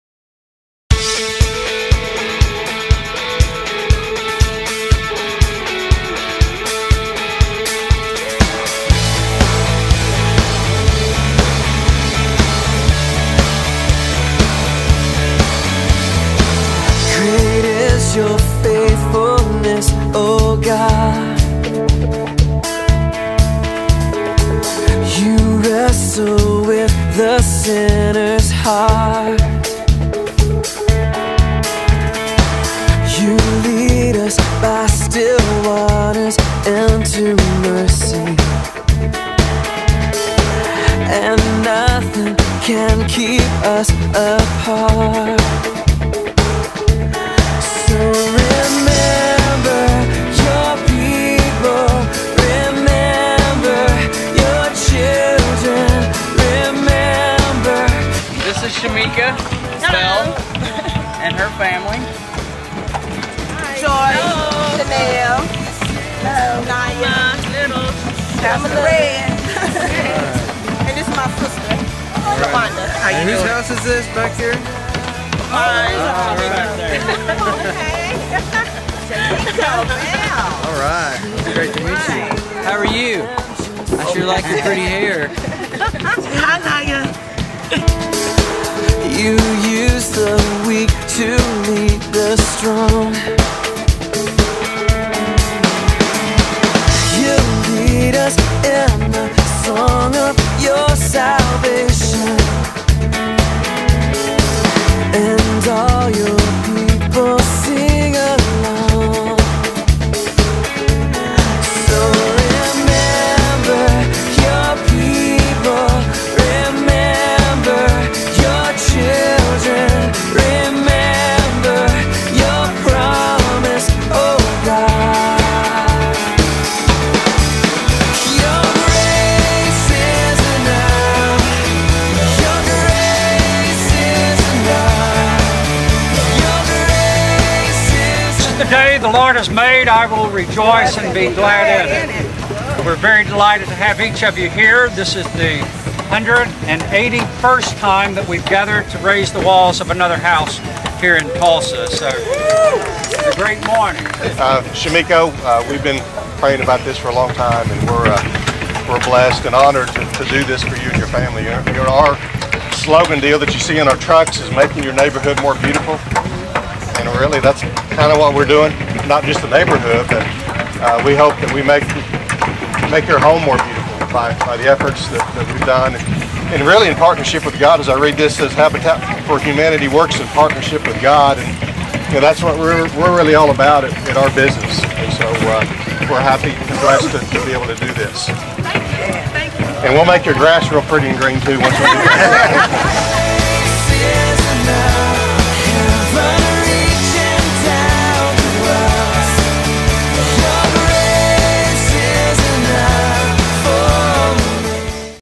Lawn America Habitat for Humanity Wall Raising
The walls were raised on Sep 6, 2008 on the Lawn America sponsored Habitat for Humanity house in N. Tulsa. It was a great ceremony, and many people came out to support our crew of volunteers. Big Jake also came out for the event and helped out!